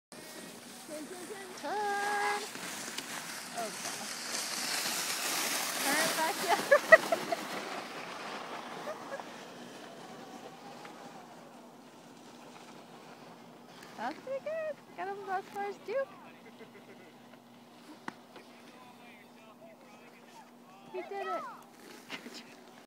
Fwd: Keith sledding
Keith giving the runner sled a try.  Could have gotten more speed if he kept his feet up,